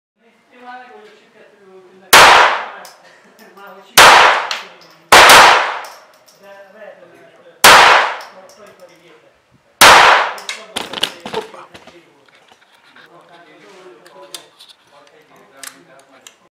Shooting Morini from close
Shooting with Morini CM 22M RF Semi-Auto Match Pistol